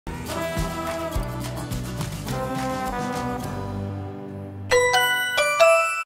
اكتشفوا فوائد لافاش كيري® العديدة: الحديد
كل قطعة لافاش كيري® كاتساهم ب 4.5% من الكمية اليومية الموصى بها من الحديد، للأطفال ما بين 7 و10 سنوات! 😁 فنظركم شنو هي الفوائد ديالو؟ أجيو نكْتاشفوها فهاد الفيديو 😉👇